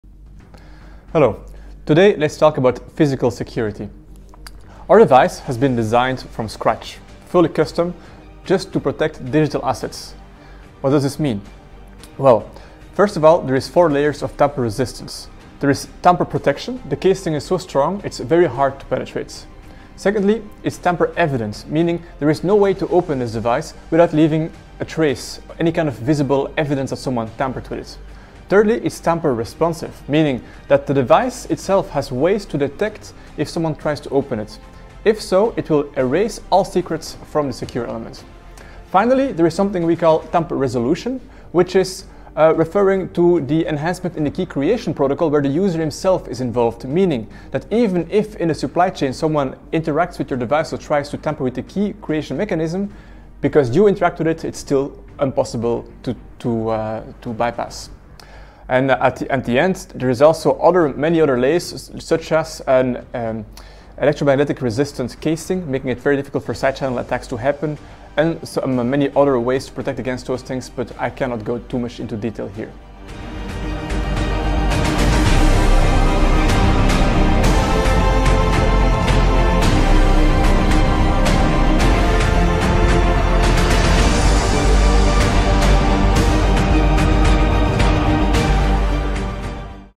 [ES] NGRAVE Sneak Peeks - Episodio 6: El NGRAVE ZERO es extremadamente seguro contra ataques físicos
En este video, el CTO de NGRAVE, Xavier Hendrickx, brinda más detalles sobre cómo la billetera de hardware ZERO de NGRAVE está construida para resistir una amplia gama de ataques físicos.

Para leer más sobre esto, consulte nuestro documento técnico / white paper, que se puede encontrar en el sitio web